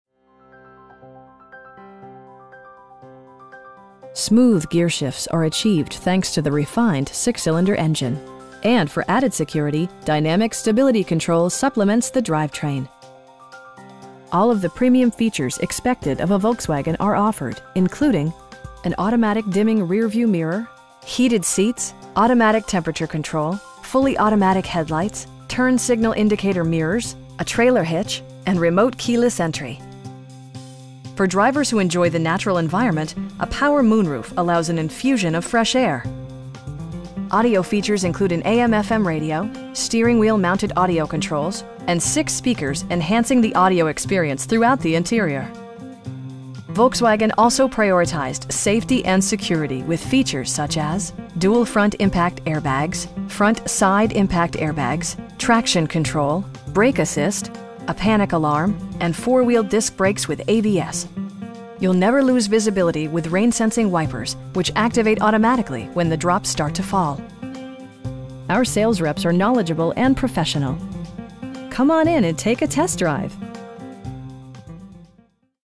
2021 Volkswagen Atlas Cross Sport in Concord, NC 28027
7500 Hendrick Auto Plaza NW

Smooth gearshifts are achieved thanks to the refined 6 cylinder engine, and for added security, dynamic Stability Control supplements the drivetrain. All of the premium features expected of a Volkswagen are offered, including: an automatic dimming rear-view mirror, heated seats, automatic temperature control, fully automatic headlights, turn signal indicator mirrors, a trailer hitch, and remote keyless entry. For drivers who enjoy the natural environment, a power moon roof allows an infusion of fresh air. Audio features include an AM/FM radio, steering wheel mounted audio controls, and 6 speakers, enhancing the audio experience throughout the interior. Volkswagen also prioritized safety and security with features such as: dual front impact airbags, front side impact airbags, traction control, brake assist, a panic alarm, and 4 wheel disc brakes with ABS. You'll never lose visibility with rain sensing wipers, which activate automatically when the drops start to fall. Our sales reps are knowledgeable and professional. We'd be happy to answer any questions that you may have. Come on in and take a test drive!